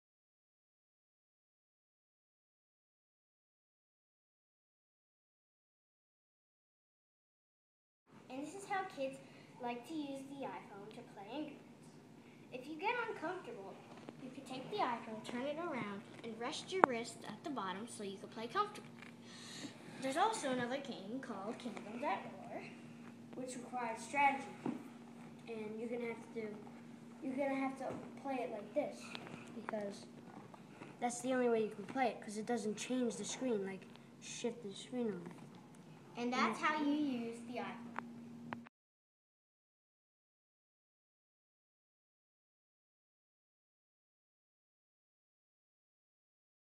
Kids use iPads
Kids demonstrate how they like to use iphome.